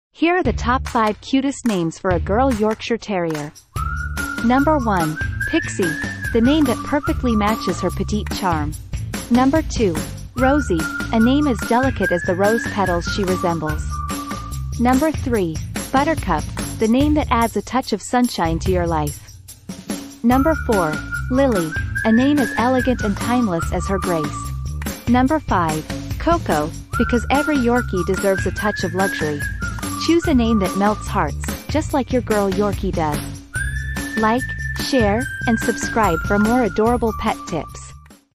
Top 5 Cutest Names for a Girl Yorkshire Terrier
Are you ready to meet the most adorable Yorkshire Terrier names? In this delightful video, we unveil the top 5 cutest names for your girl Yorkshire Terrier, guaranteed to steal your heart.

Whether you're a proud Yorkshire Terrier parent or just an admirer of these charming pups, you won't want to miss this. These names aren't just labels; they're reflections of your Yorkie's unique personality and style.

So, which name will best suit your Yorkshire Terrier? Dive into this video to find the perfect match for your furry friend.

Discover the magic of Yorkshire Terrier names with us. Your pint-sized companion deserves a name as special as she is. Join the fun and embark on this naming adventure today!